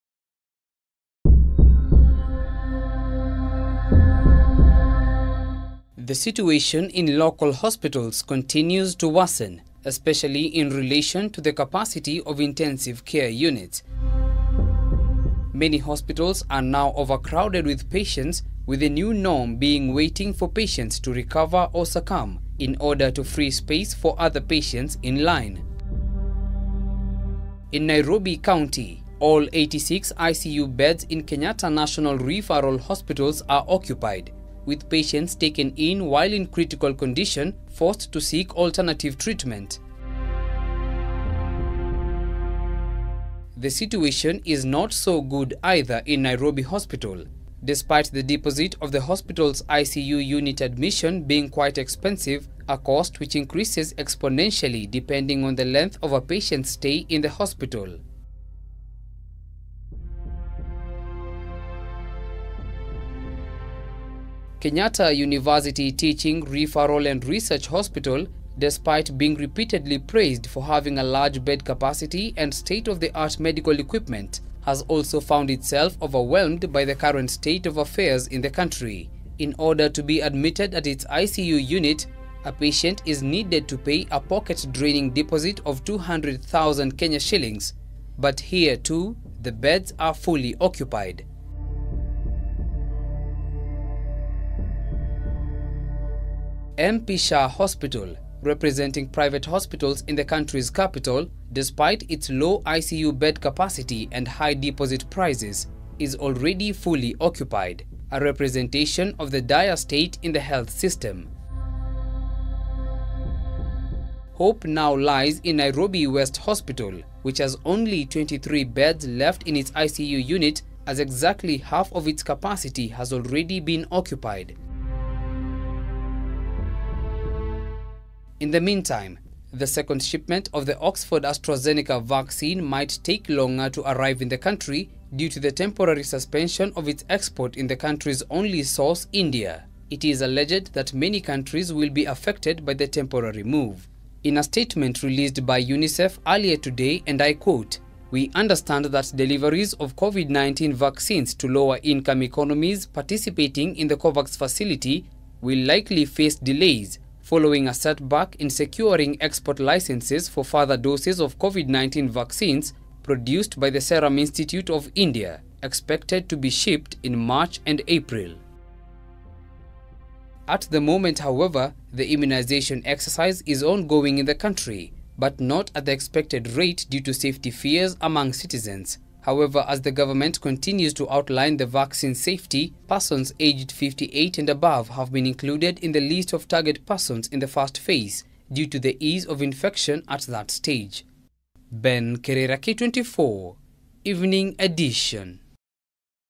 Crisis in Kenya as capital's ICU capacity strains to accomodate increasing patients
Many hospitals are now in Nairobi are overcrowded with patients, with the new norm being waiting for patients to or recover or succumb in order to free space for other patients in line.